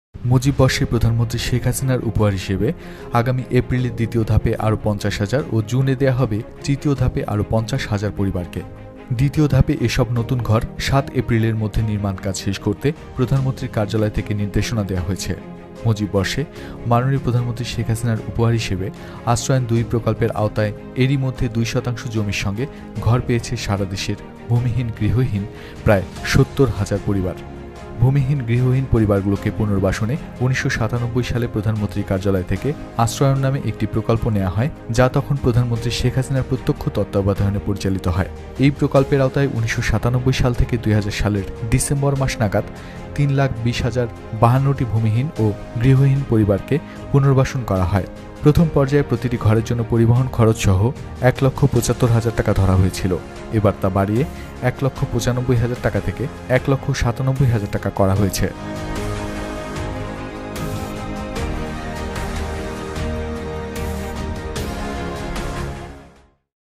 মুজিববর্ষ উপলক্ষে চলতি বছরেই ঘর পাবে আরও ১ লাখ পরিবার
মুজিববর্ষে প্রধানমন্ত্রী শেখ হাসিনার উপহার হিসেবে আগামী এপ্রিলে দ্বিতীয় ধাপে আরও ৫০ হাজার ও জুনে দেয়া হবে তৃতীয় ধাপের আরও ৫০ হাজার পরিবারকে। দ্বিতীয় ধাপের এসব নতুন ঘর ৭ এপ্রিলের মধ্যে নির্মাণ কাজ শেষ করতে প্রধানমন্ত্রীর কার্যালয় থেকে নির্দেশনা দেয়া হয়ছে। মুজিববর্ষে মাননীয় প্রধানমন্ত্রী শেখ হাসিনার উপহার হিসেবে আশ্রয়ণ-২ প্রকল্পের আওতায় এরইমধ্যে ২ শতাংশ জমির সঙ্গে ঘর পেয়েছে সারা দেশের ভূমিহীন-গৃহহীন প্রায় ৭০ হাজার পরিবার। ভূমিহীন-গৃহহীন পরিবারগুলোকে পুনর্বাসনে ১৯৯৭ সালে প্রধানমন্ত্রীর কার্যালয় থেকে আশ্রয়ণ নামে একটি প্রকল্প নেয়া হয় যা তখন প্রধানমন্ত্রী শেখ হাসিনার প্রত্যক্ষ তত্ত্বাবধানে পরিচালিত হয়। এই প্রকল্পের আওতায় ১৯৯৭ সাল থেকে ২০২০ সালের ডিসেম্বর মাস নাগাদ ৩ লাখ ২০ হাজার ৫২টি ভূমিহীন ও গৃহহীন পরিবারকে পুনর্বাসন করা হয়। প্রথম পর্যায়ে প্রতিটি ঘরের জন্য পরিবহন খরচসহ ১ লাখ ৭৫ হাজার টাকা ধরা হয়েছিল। এবার তা বাড়িয়ে ১ লাখ ৯৫ হাজার টাকা থেকে ১ লাখ ৯৭ হাজার টাকা করা হয়েছে।